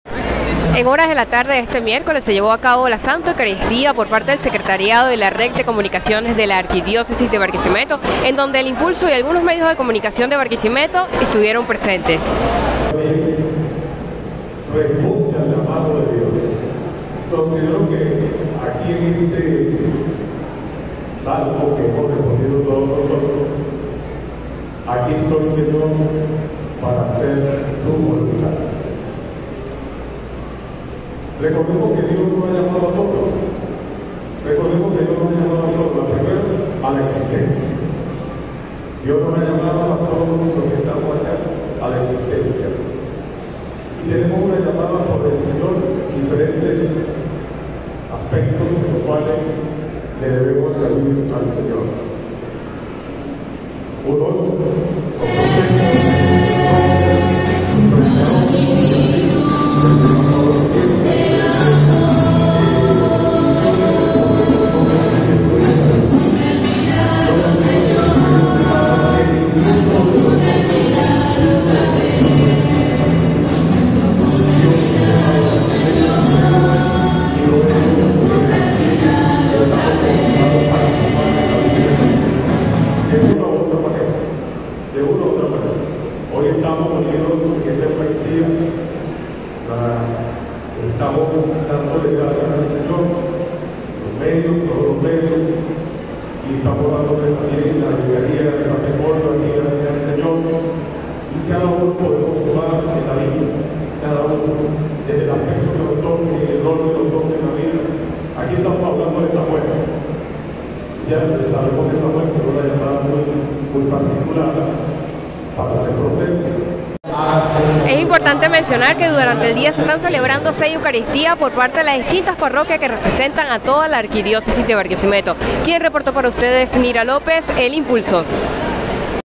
AvanceIMP Se llevó a cabo en Santa Rosa misa en honor a los medios de comunicación de Lara
Durante la tarde de este miércoles se lleva a cabo una misa por parte del Secretariado de la Red de Comunicaciones de la Arquidiócesis de Barquisimeto. Varios medios de comunicación, incluyendo El Impulso, se encuentran presentes en la iglesia de Santa Rosa.

Durante la ceremonia, se hizo énfasis en la importancia de servir a Dios en el mundo actual.

«Dios nos ha llamado a todos los que estamos acá a la existencia. Y tenemos que hablar con el Señor de diferentes aspectos con los cuales debemos servirle», expresó el sacerdote que oficiaba la Eucaristía.

Es importante mencionar que durante el día se están celebrando 6 eucaristías por parte de las distintas parroquias que representan a la Arquidiócesis de Barquisimeto.

Cabe resaltar que esta actividad forma parte del cronograma establecido en el contexto de la visita 166 de la Divina Pastora a Barquisimeto.

Texto: Enrique Suárez
Video: Nira López